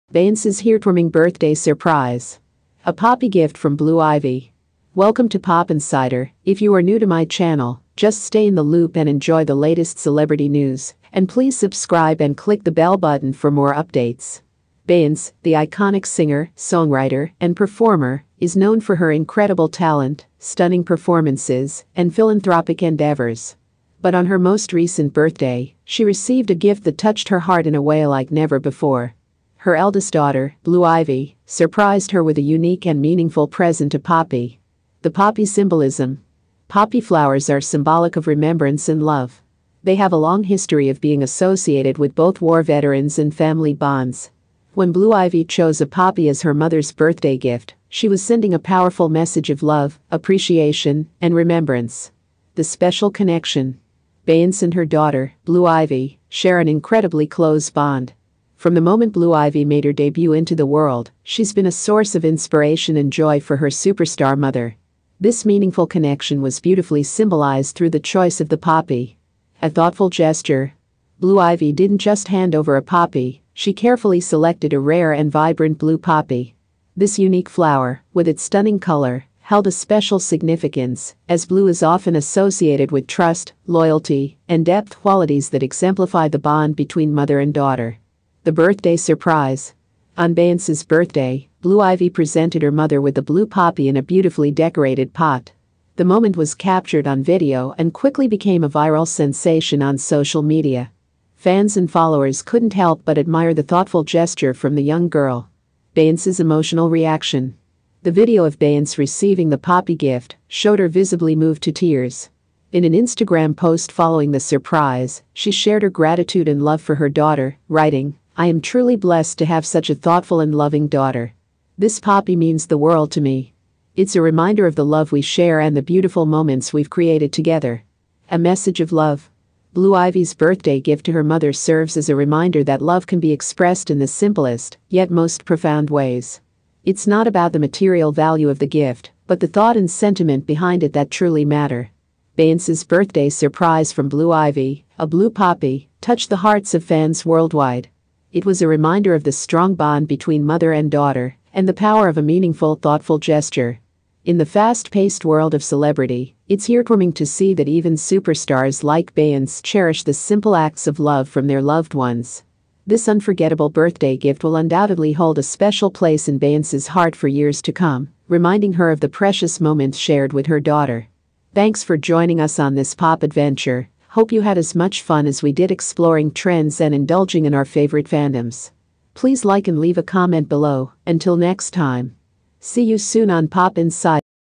"Beyonce's Heartwarming Birthday Surprise: A Poppy Gift from Blue 💙 Ivy".
Beyoncé, the iconic singer, songwriter, and performer, is known for her incredible talent, stunning performances, and philanthropic endeavors. But on her most recent birthday, she received a gift that touched her heart in a way like never before.Her eldest daughter, Blue Ivy, surprised her with a unique and meaningful present  a poppy.